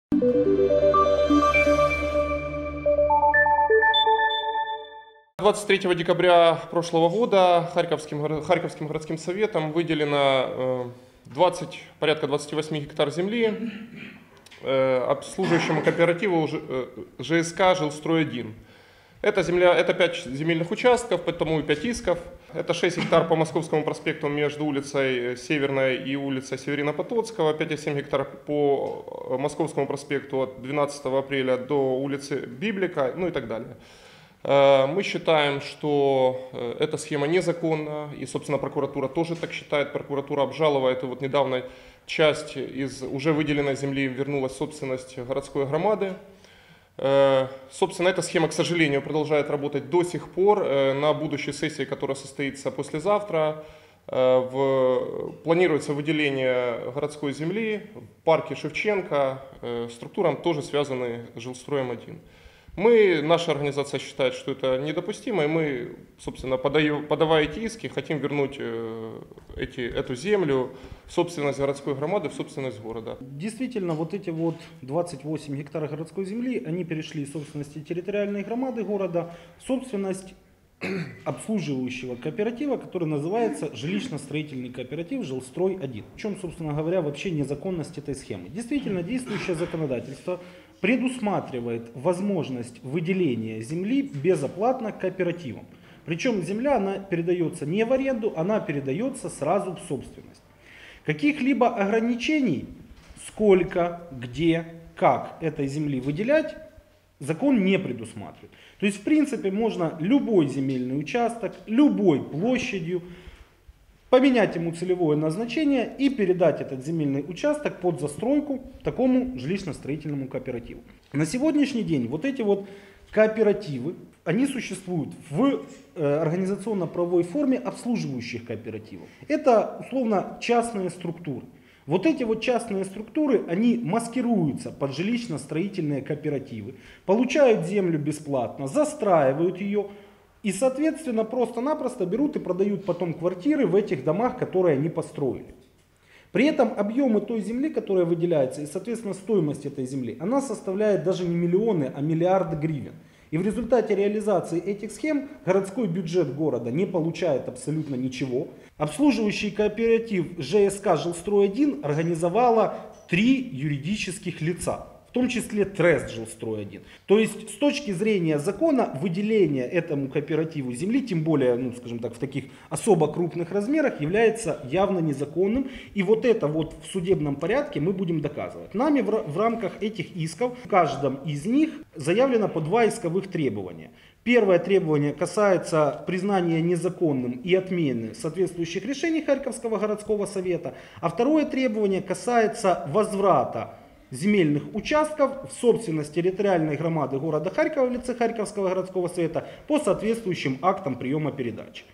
Требование вернуть 28 га земли от харьковских общественников
Общественная организация "Харьковский антикоррупционный центр" подала 5 исков в хозяйственный суд Харьковской области с требованием вернуть в собственность территориальной общины города 6 земельных участков общей площадью 28 га, которые сейчас находятся в собственности ЖСК " Жилстрой-1 ".
Ранее подобные иски по другим аналогичным решениям Харьковского горсовета подавала прокуратура.